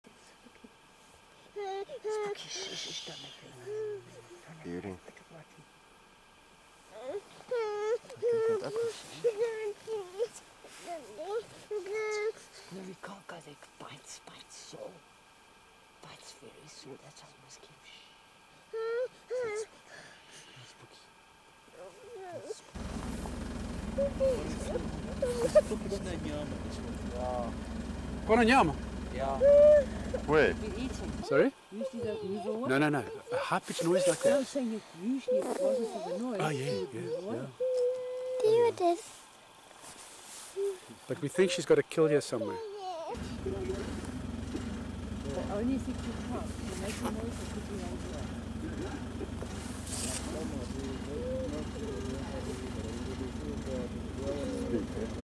leopard night game drive kruger park south africa
On a night-time game drive near Kruger Park a little girl kept whining, which attracted a little too much attention from prowling leopards.